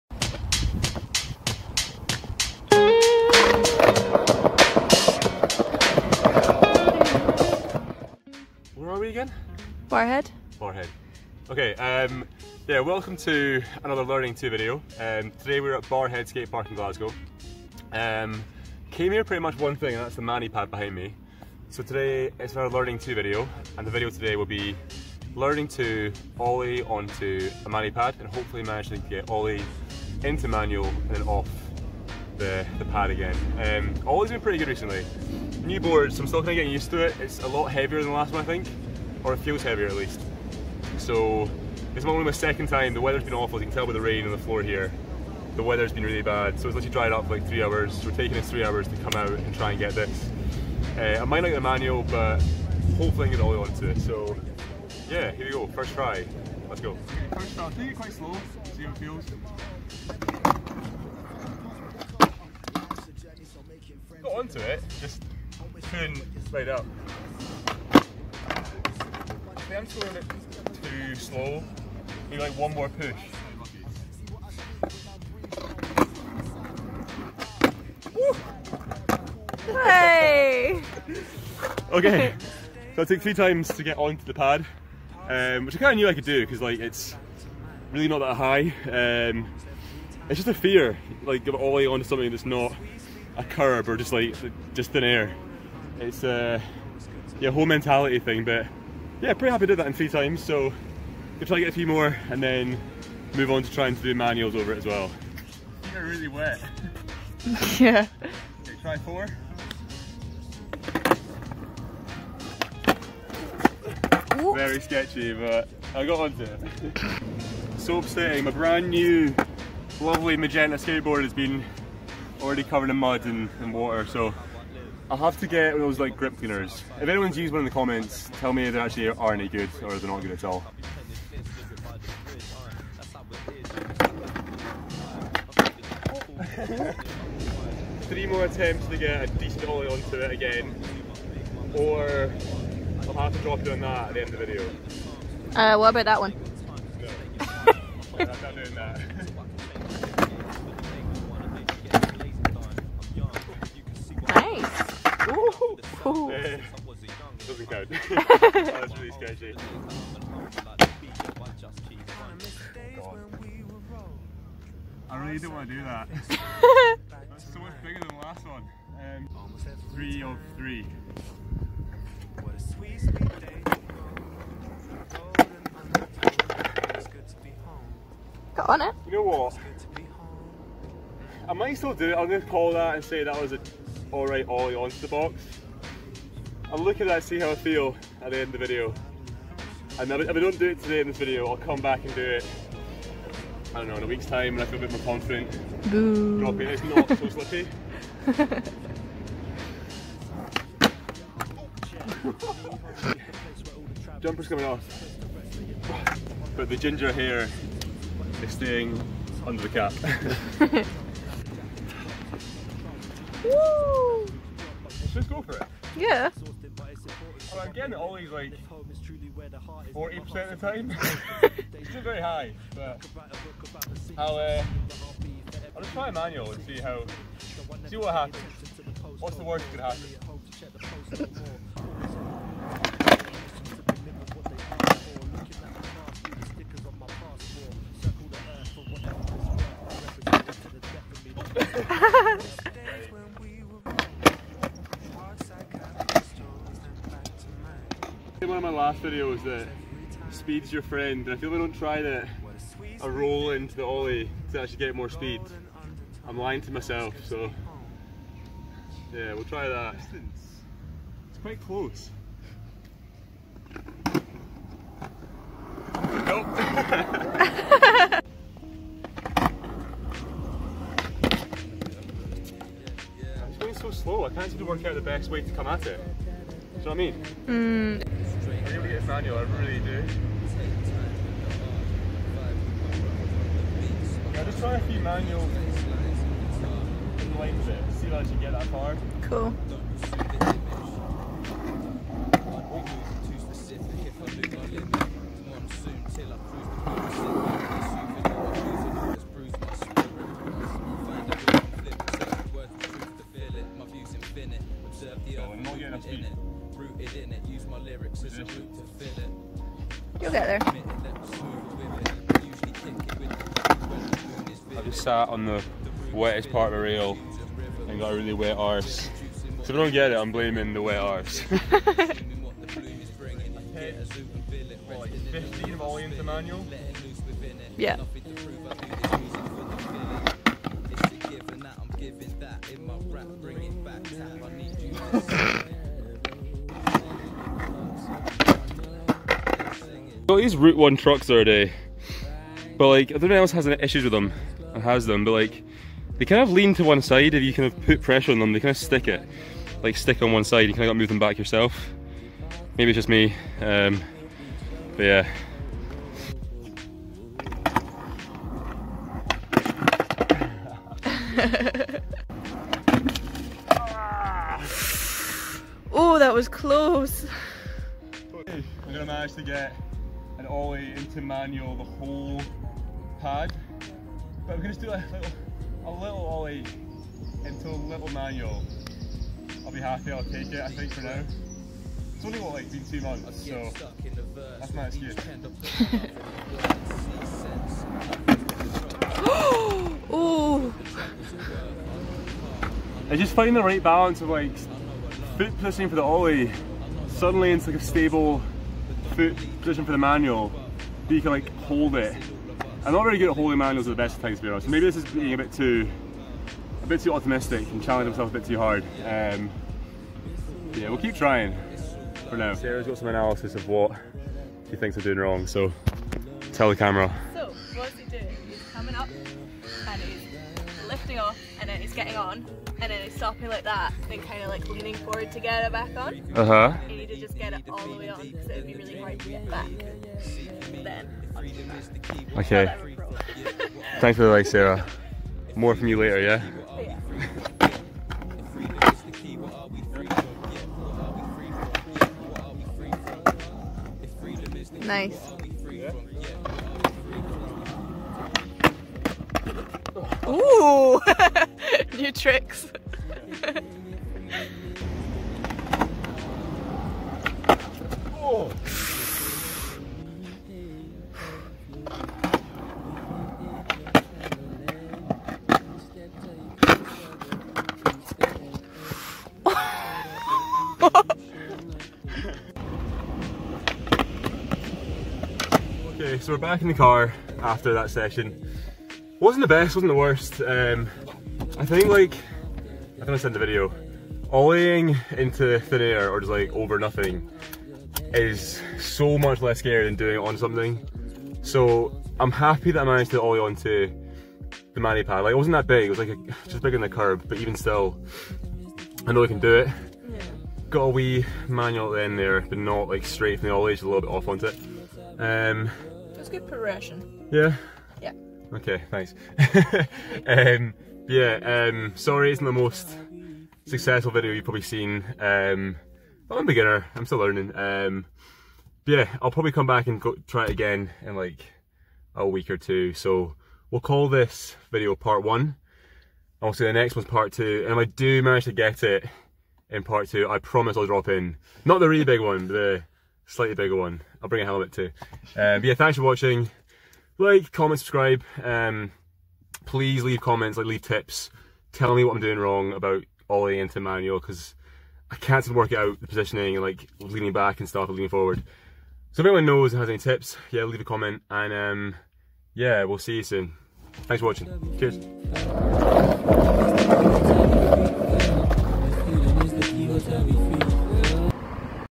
Learning to OLLIE INTO MANUAL (Part 1) | Learning to Skateboard
Today's Learning To video focused on me trying to work on ollieing onto an object and get into a manual. I've been doing a lot of ollies recently, but haven't found anywhere nearby where I could practise ollieing onto an object, it's a lot scarier.

Didn't get the manual down in this video so I'll be uploading part 2 next week when I can get back to this skatepark!

Cheers.